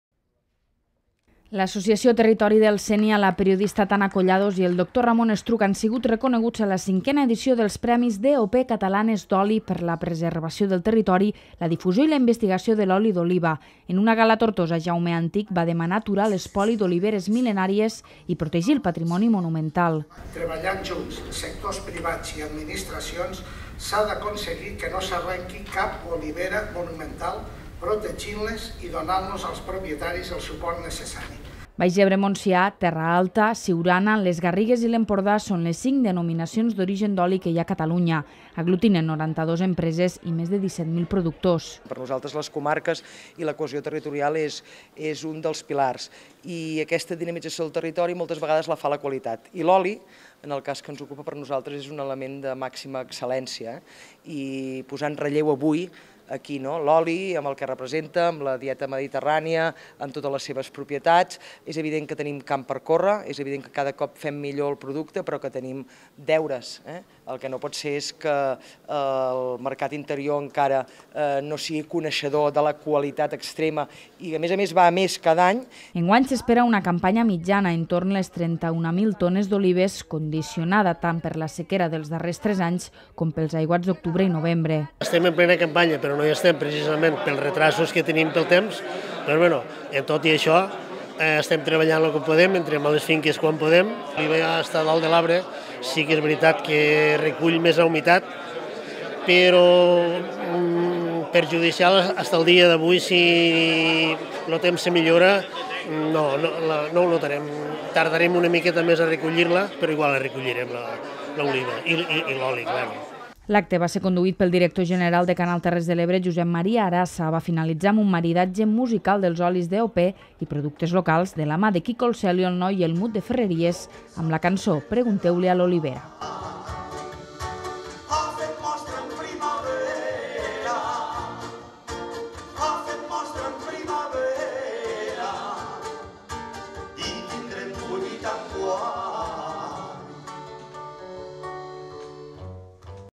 Lliurats a Tortosa els premis DOP Catalanes d'Oli
L’associació Territori del Sénia, la periodista Tana Collados i el doctor Ramon Estruch han sigut reconeguts en la cinquena edició dels Premis DOP Catalanes d’Oli. L’ha presidit la consellera d’Agricultura, Teresa Jordà, en un acte a Tortosa.